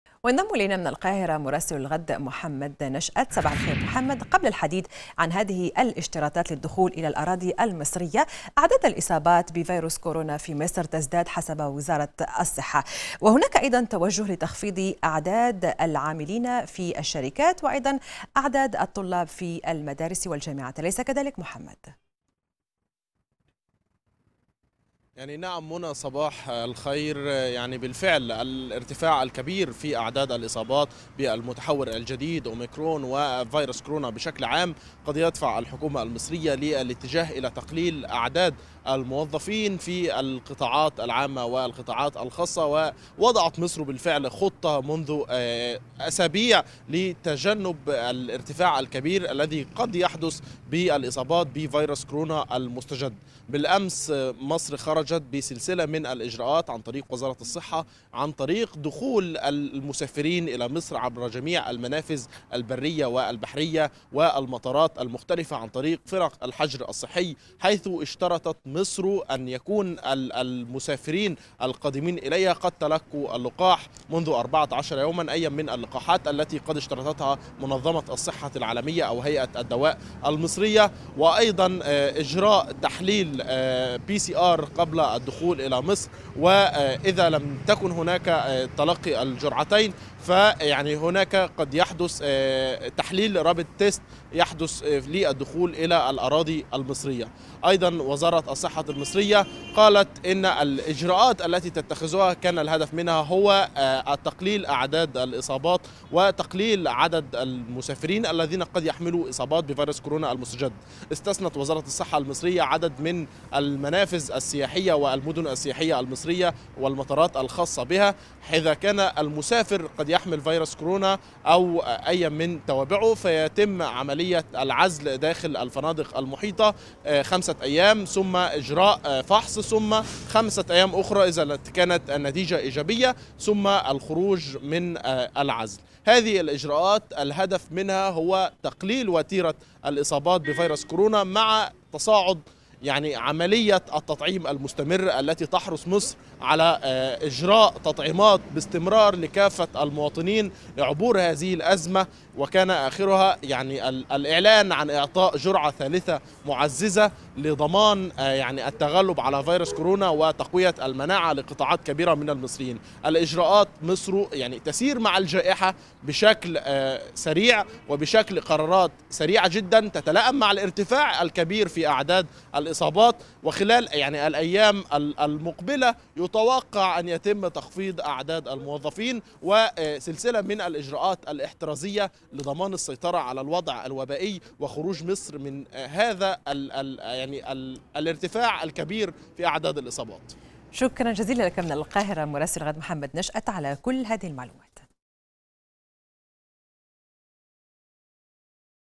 مصر | إجراءات جديدة لمواجهة انتشار فيروس كورونا
قناة_الغد | تطبق في مصر بدءا من اليوم اشتراطات صحية جديدة على منافذ دخول البلاد الجوية والبحرية والبرية تزامنا مع المستجدات الخاصة بفيروس كورونا.
وبحسب وزارة الصحة، فإن هذه الإجراءات تضمنت اشتراط حصول القادمين إلى البلاد سواء من المصريين أو غير المصريين، على أي من اللقاحات المضادة لفيروس كورونا، شرط مرور أربعة عشر يوما على التطعيم، مشيرة إلى أنه في حال عدم وجود شهادة تفيد تلقي اللقاح، فيشترط أن يكون القادم للبلاد لديه شهادة تفيد سلبيته لفيروس كورونا.. مزيد من التفاصيل يتحدث من القاهرة مراسل الغد محمد نشأت.